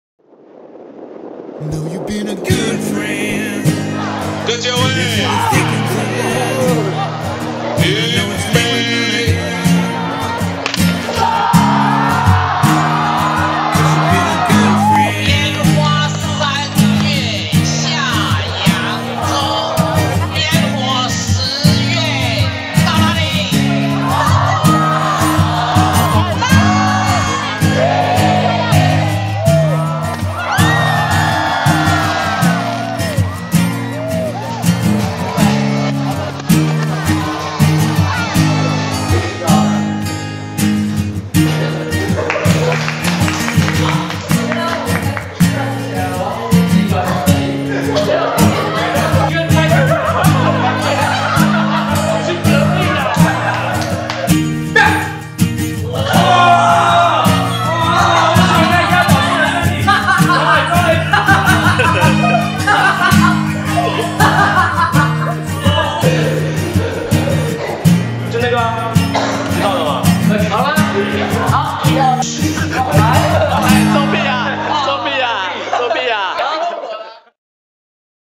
26th Anniversary of National Chung Cheng University
This is a trailer about my school festival and my youtube channel. I use YouTube and Blogger to record my life.

I'm a draftee in Taipei now. I'll always update a 1-min film like this to tell you what my daily life is in Taiwan every month!